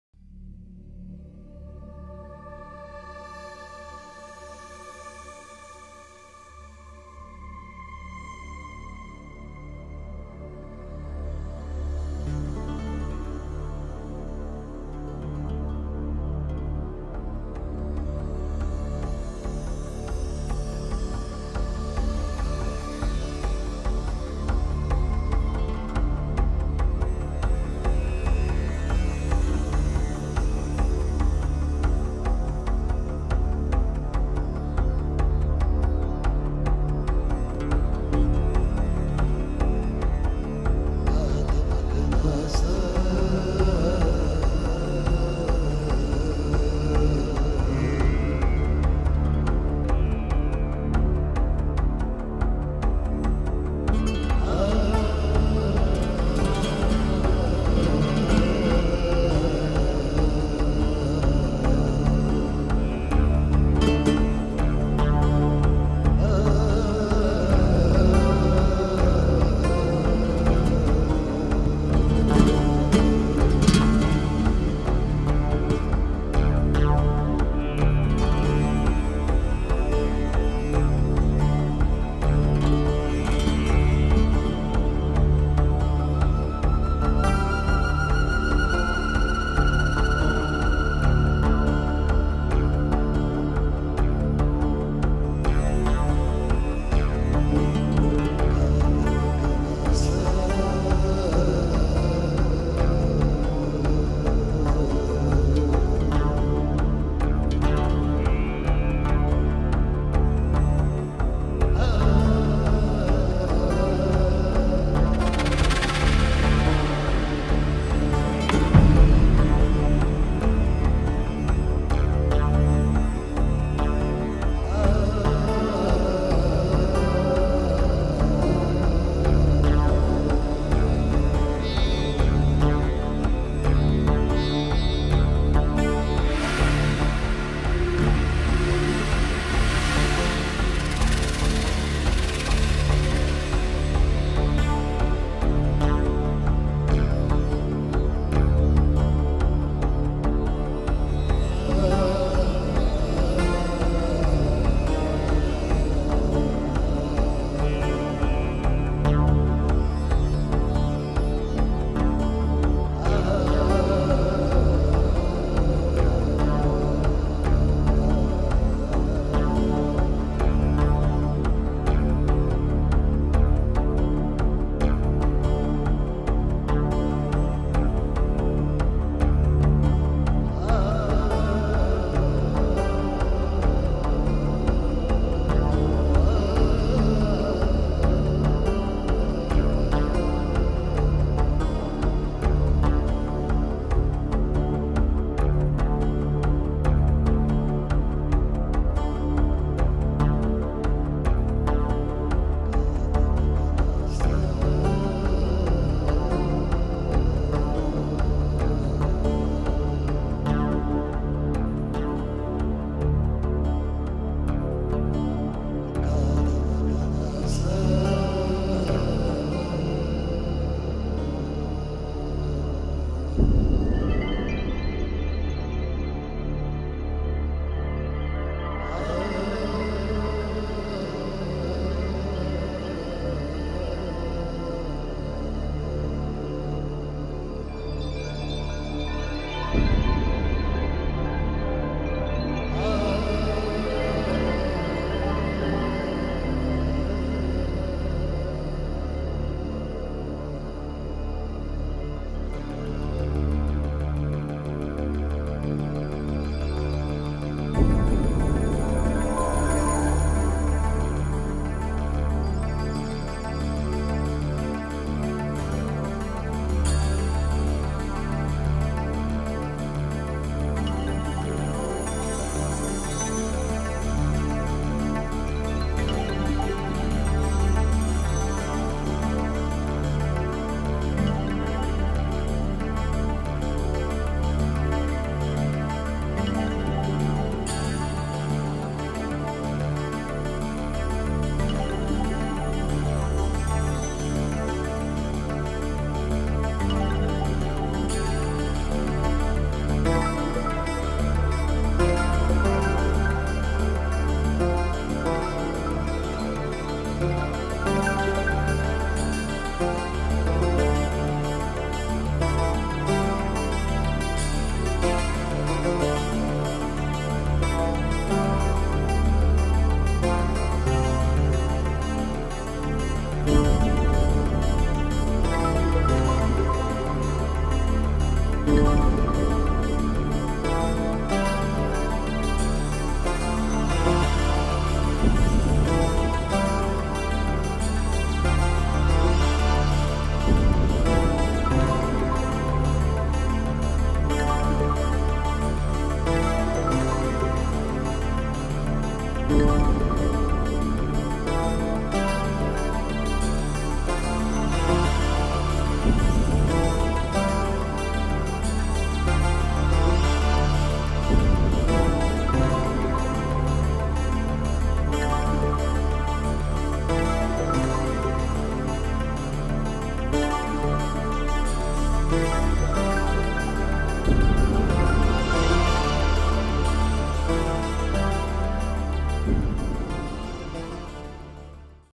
"The Terra Toreador", a painting by Raymond Pilarczyk
All imagery is property of the artist and may not be used without the artist's expressed permission. The musical score by Vangelis is used to compliment the art and not for commercial use... the art might not express the views or opinions of the recording artists